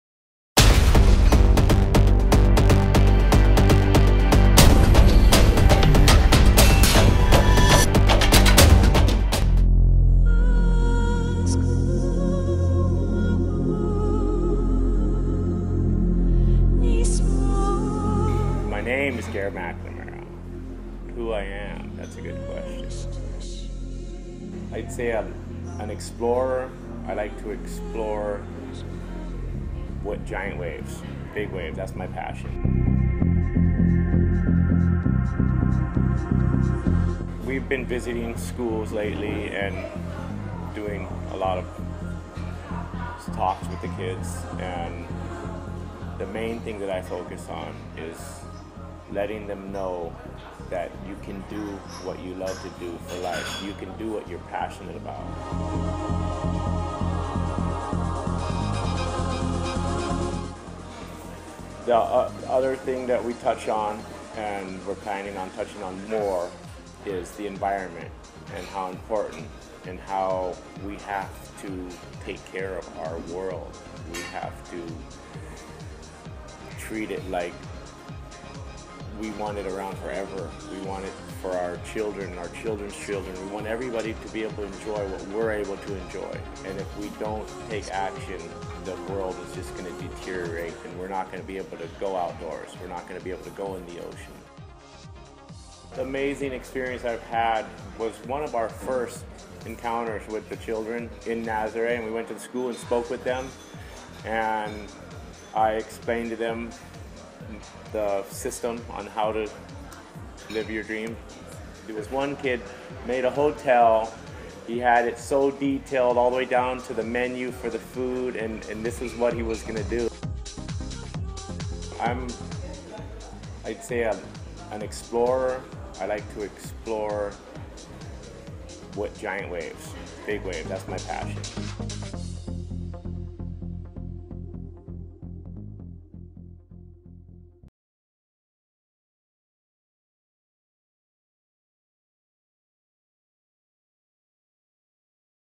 Tiny Productions - Garrett McNamara
Look in to the big wave surfer legend Garrett McNamara and his talk about to follow your passion and to take care of the environment for the future generations.